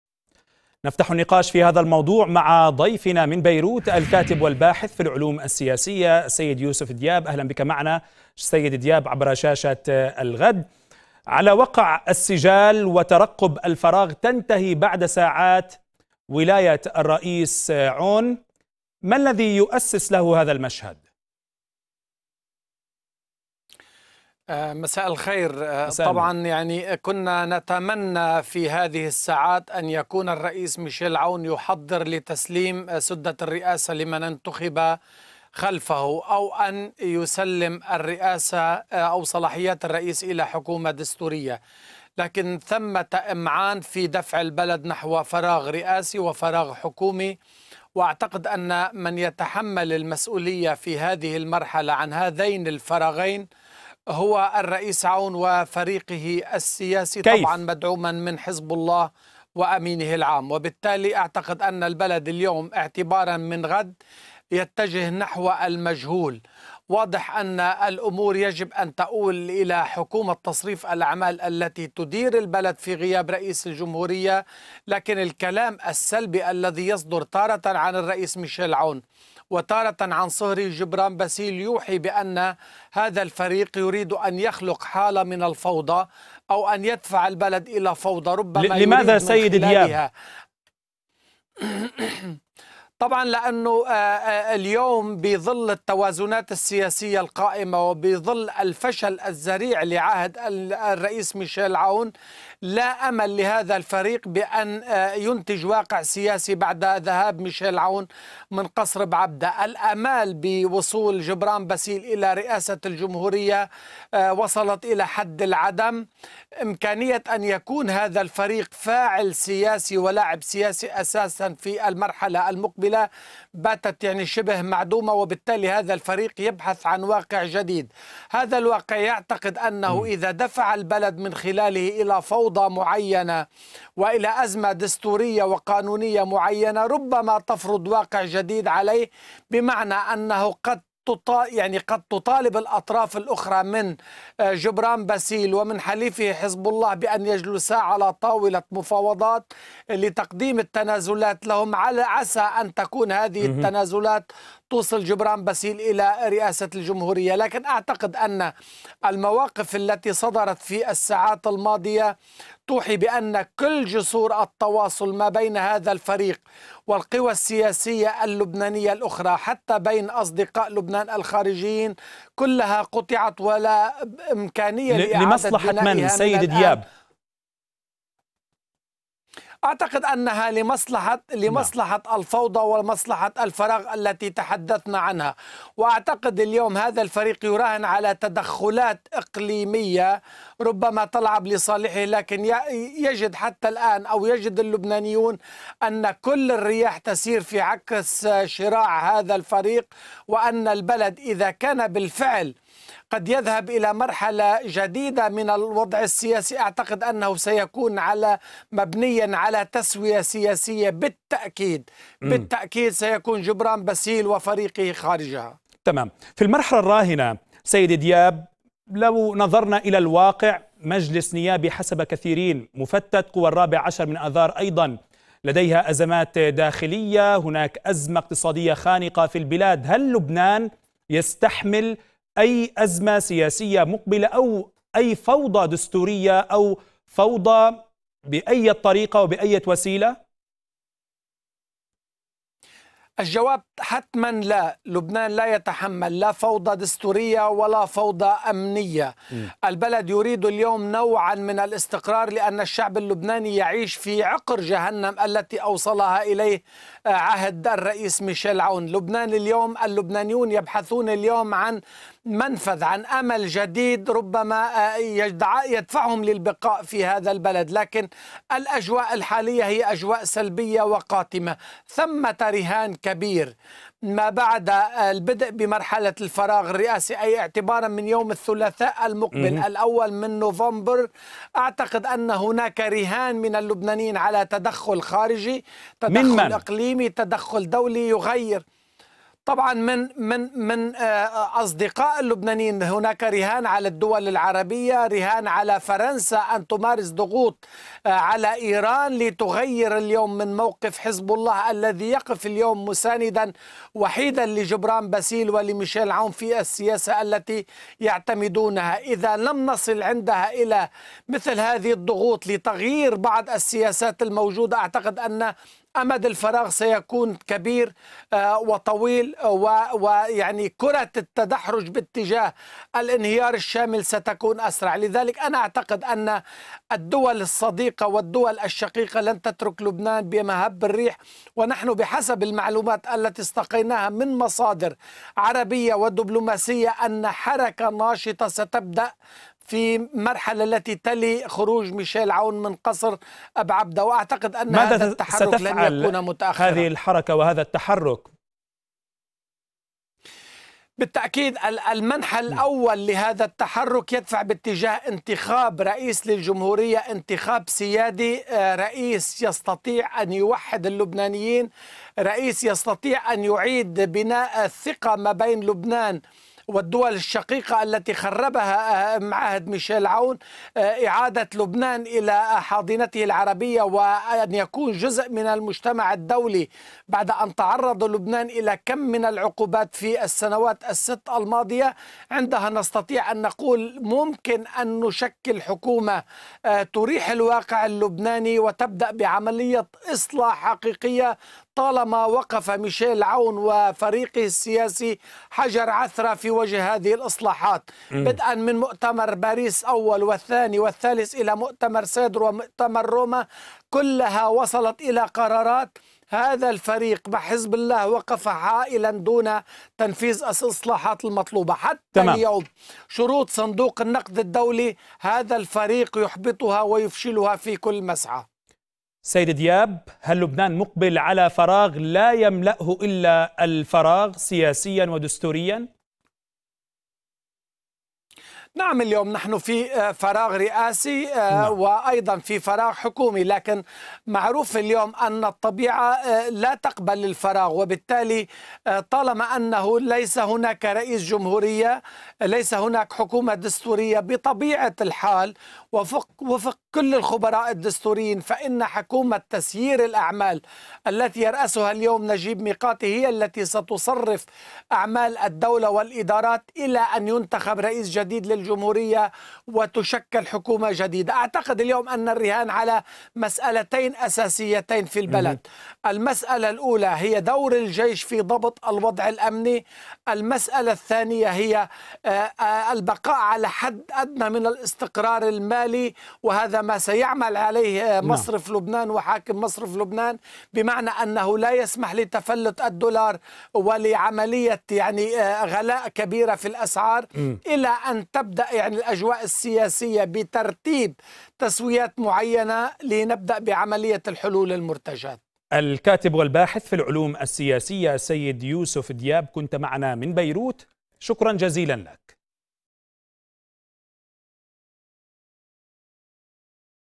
كيف يمكن تقييم عهد الرئيس اللبناني ميشال عون؟
سجال_حاد بدأ منذ تسلم الرئيس اللبناني ميشال عون مقاليدَ الحكم حول طريقة إدارته لشؤون البلاد. ومع انتهاء ولايته يتعمق السجال حول تقييم عهده، فهل حقق إنجازات؟ .. أم أظهر فشلا غير مسبوق؟ فالدلالات كانت ما شهده لبنان خلال فترة عون من أزمات لم يشهدها في تاريخه؟
تفاصيل أكثر عن الموضوع مع ضيف قناة الغد من بيروت، الكاتب والباحث في العلوم السياسية، يوسف دياب.